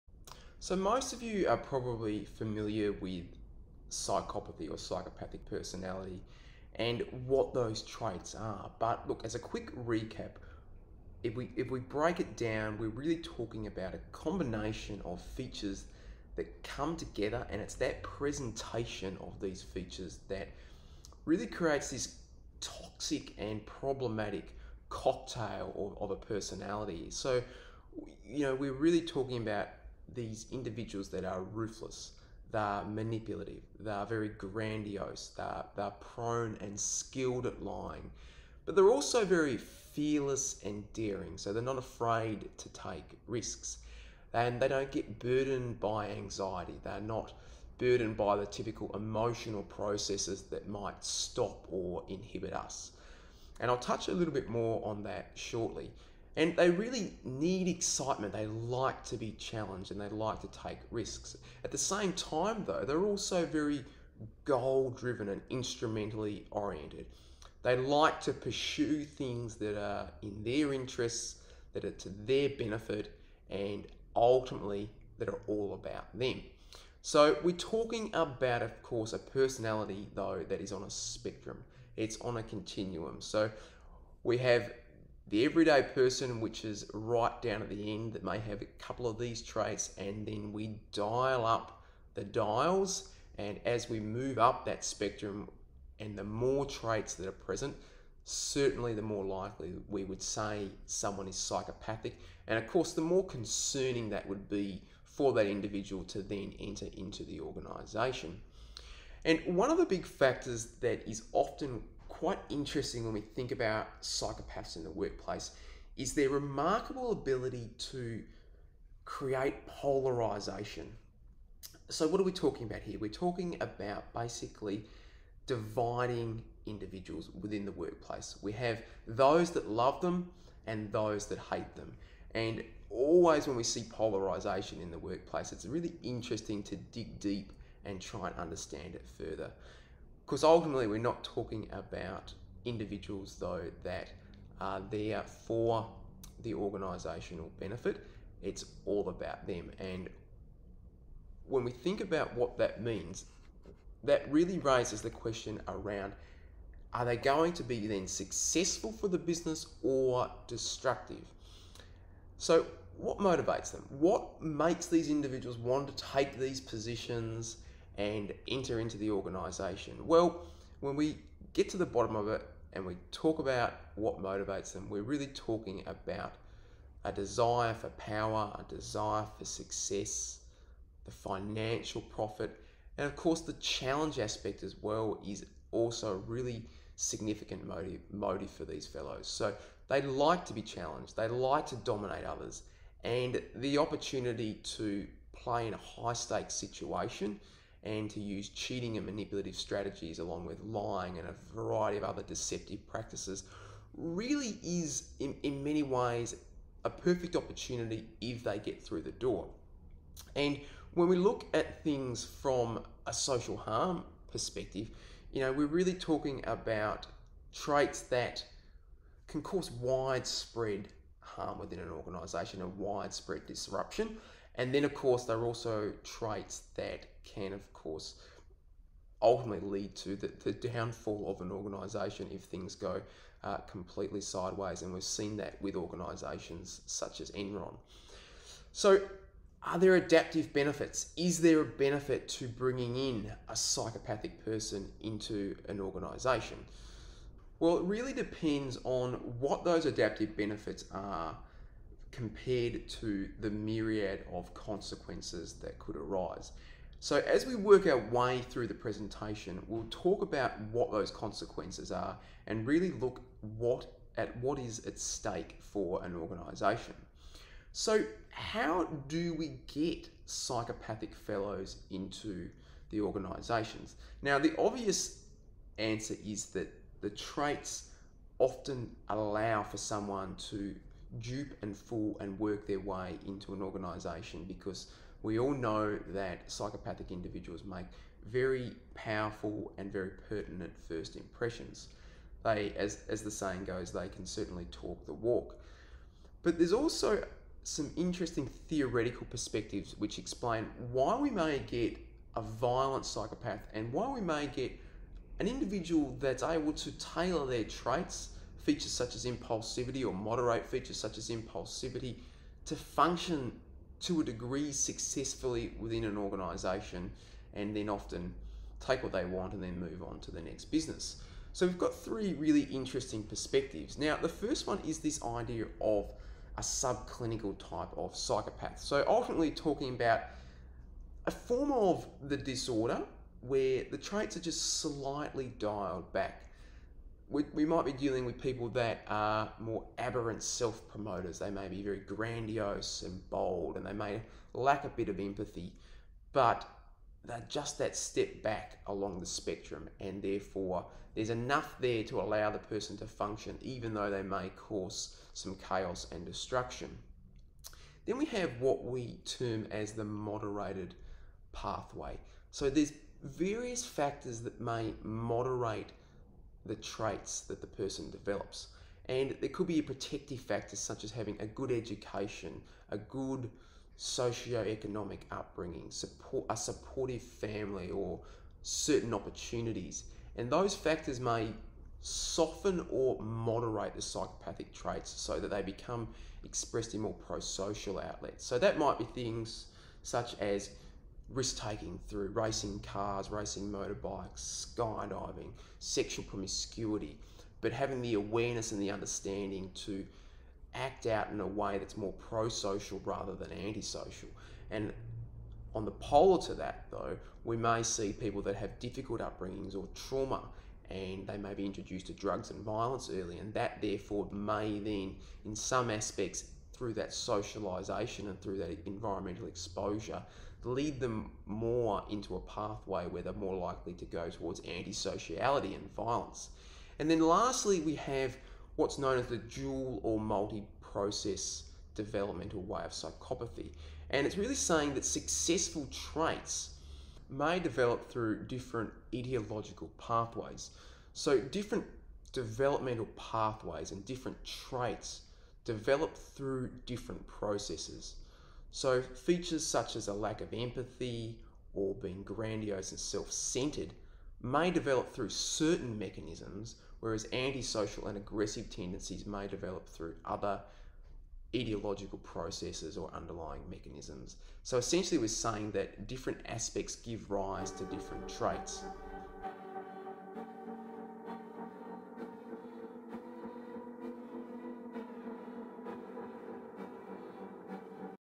What is a Corporate Psychopath?
The how and why of corporate psychopathy.